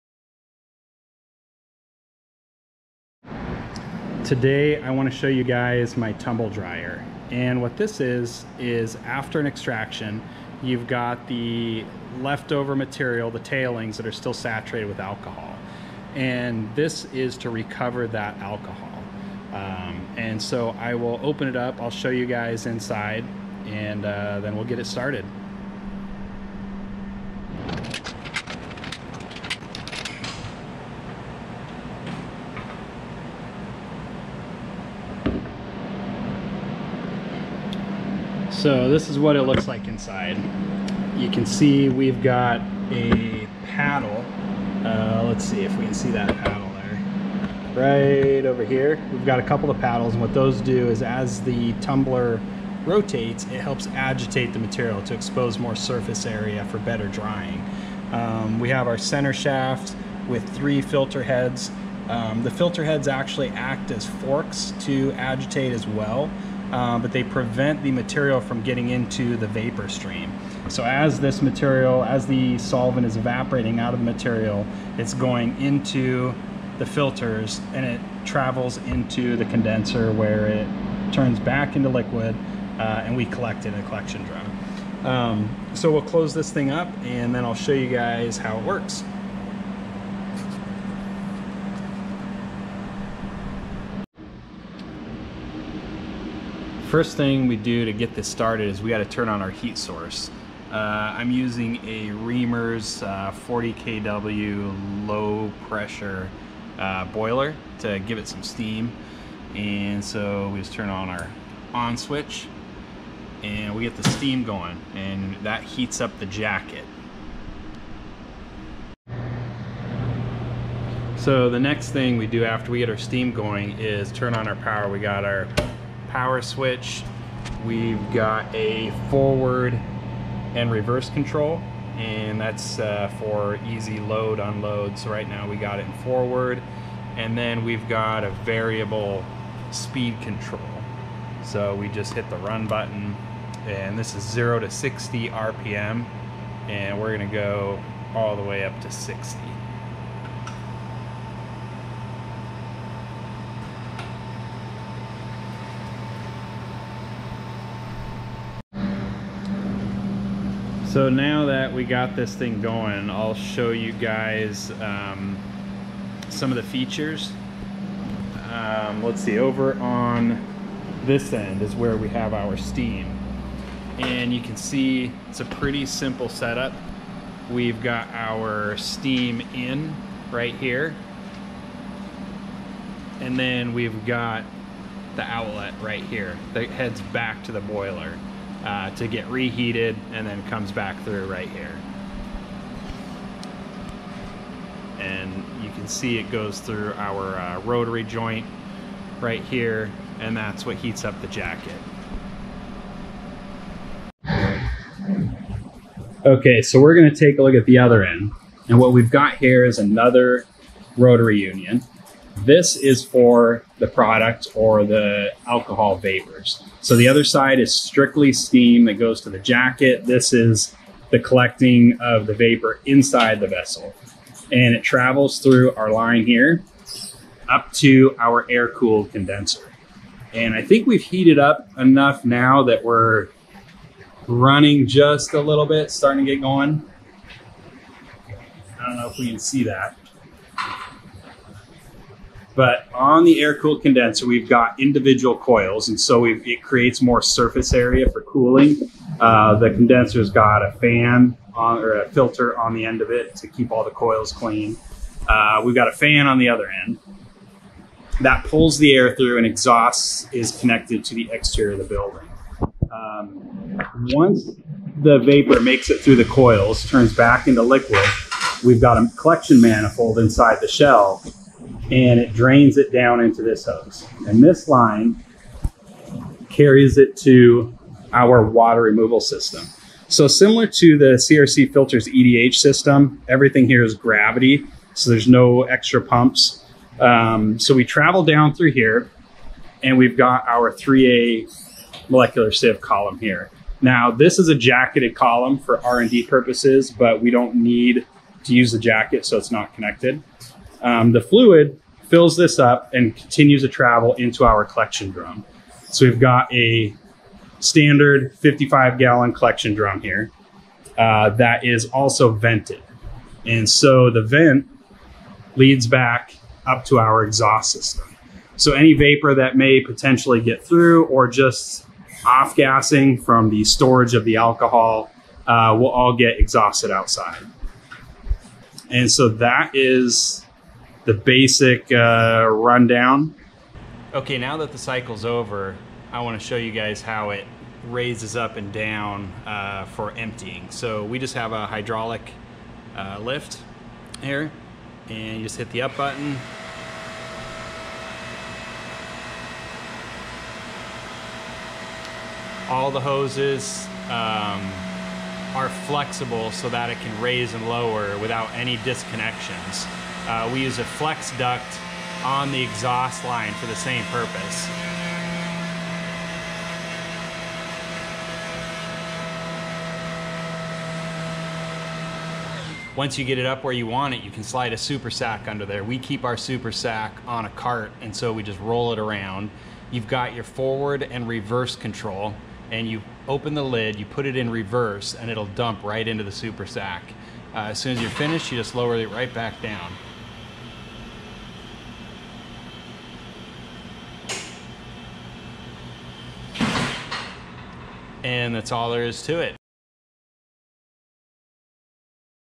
Stop using a centrifuge for cannabis processing immediately
Superior solvent recovery for hemp processors using  a tumble dryer after an ethanol extraction. Buy affordable used equipment from CRCfilters.com